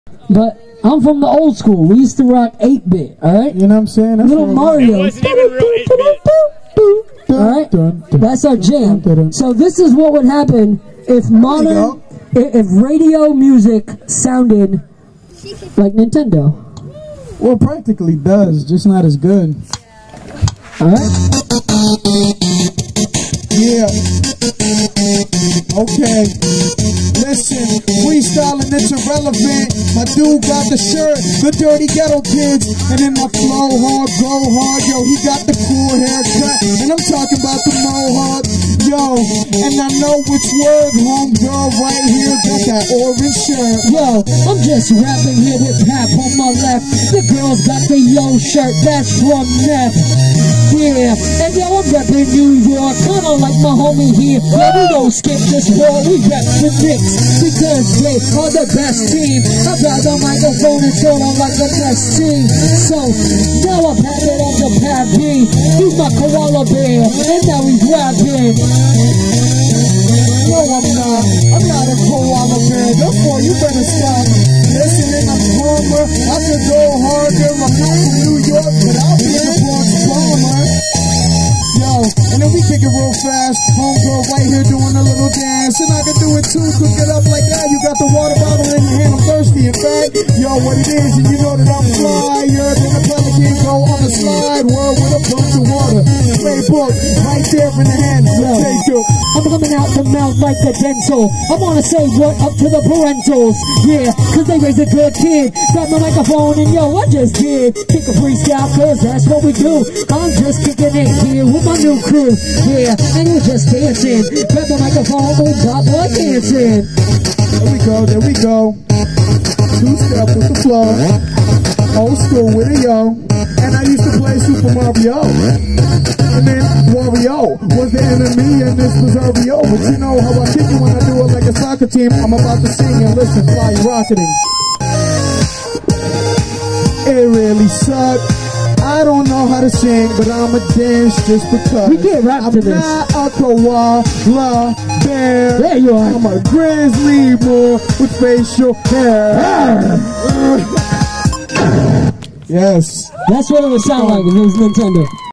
Wonder - Freestyles @ Endless Summer Bash Pt 2
Wonder & Pap perform at The Launch Youth's Endless Summer Bash Pt. 2
A sample of what modern radio would sound like as an 8bit track.